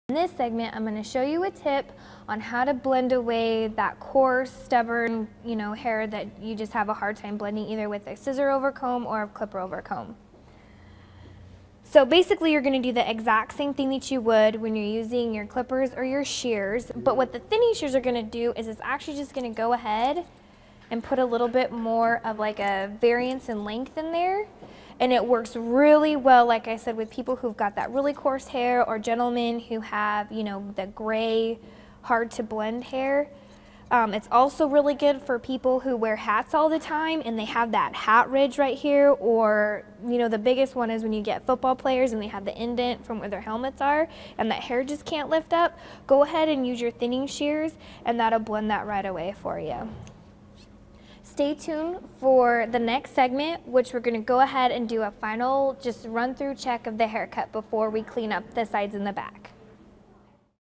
Using Thinning Shears for a Man's Clipper Haircut
Using Thinning Shears for a Man's Clipper Haircut. Part of the series: How to Do a Man's Clipper Haircut. How to use thinning shears for giving a man's haircut with clippers; get professional tips and advice on methods, techniques, and products for doing your own hairstyling in this free beauty video.